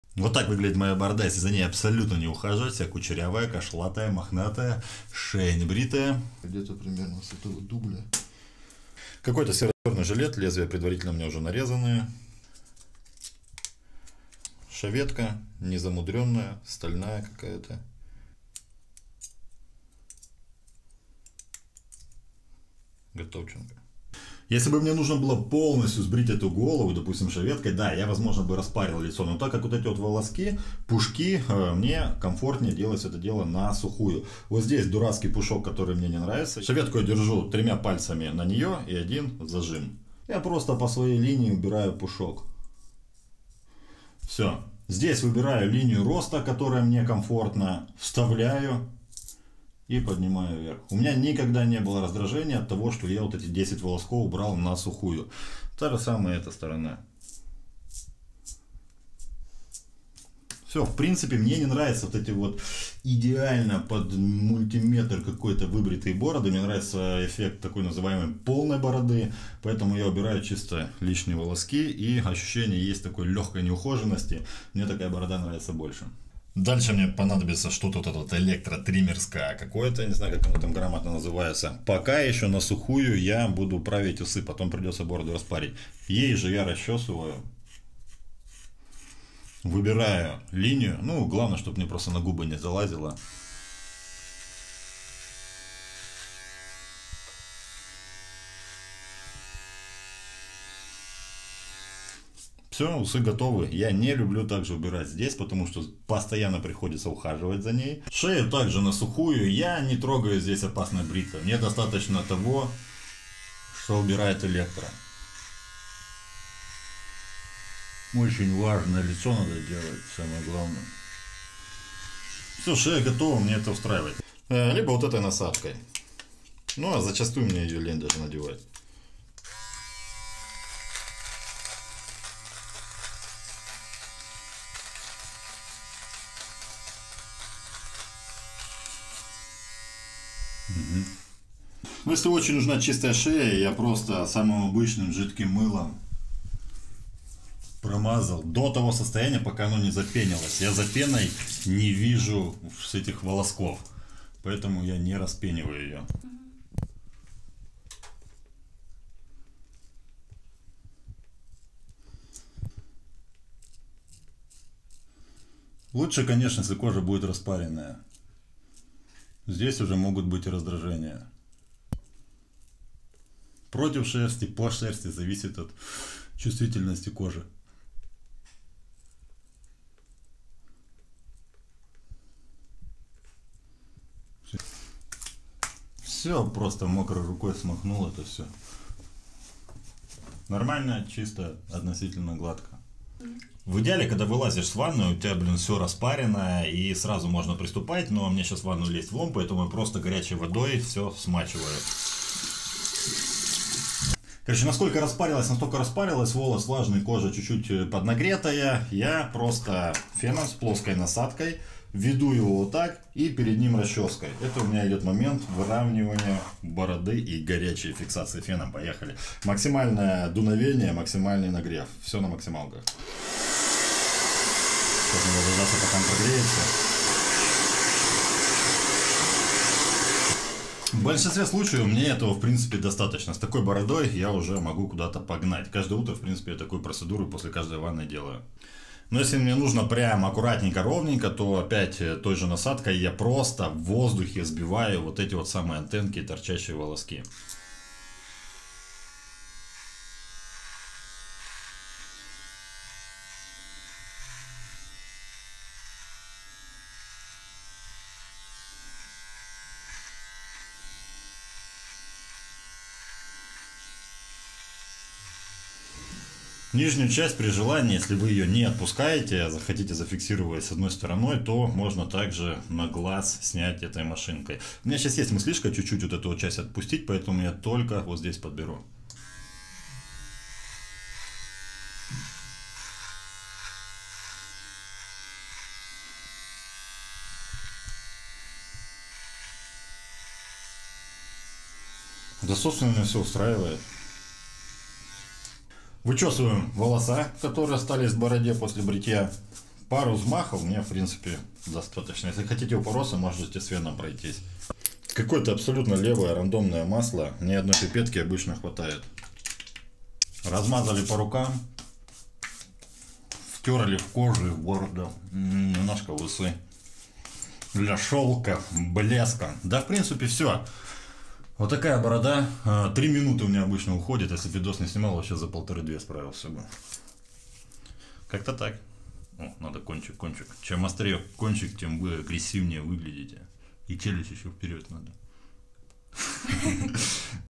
Как я оформляю бороду в домашних условиях за 5 минут
2-4 минуты и минимум вложений для того, чтобы привести бороду в порядок.

Автор канала: Владислав Носачёв
 Контакты: